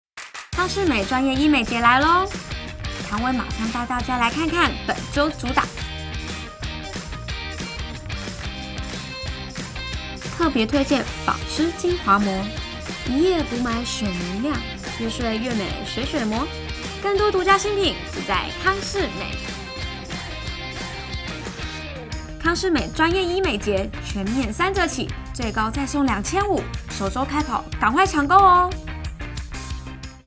康是美專業醫美節! 3/11-3/17 保養女神唐葳 告訴你內行人必買指標~首週開跑限量組合就超佛心!
第一週3/11~3/17雅漾限定新品、超值組合現省千元，首週開跑趕快搶購